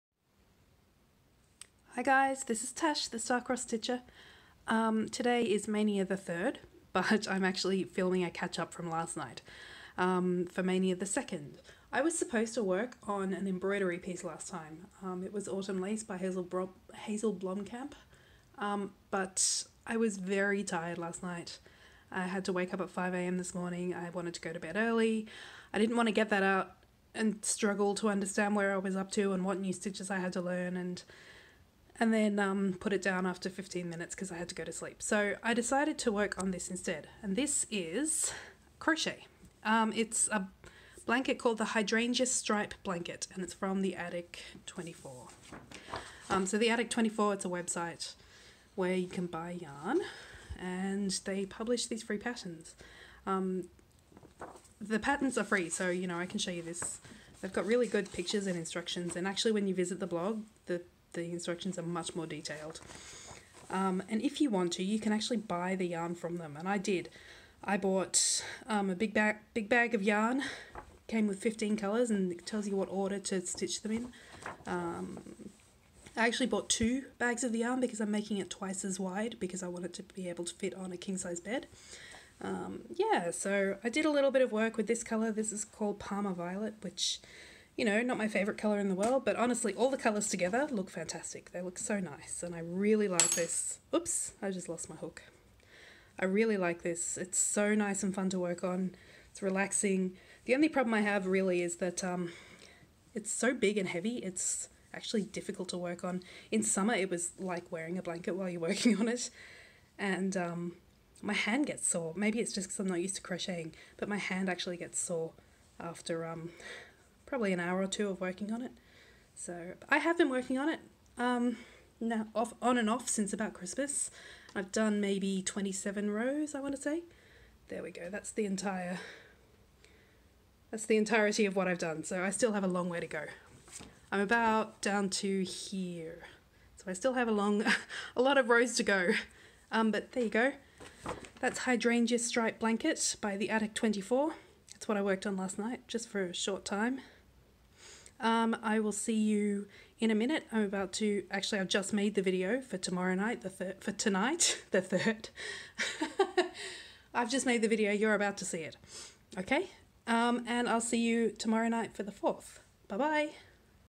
2018 Maynia the Second - Hydrangea Stripe Blanket by Attic 24
This is a crochet project, not cross stitch, sorry
The yarn pack can be purchased through that link too.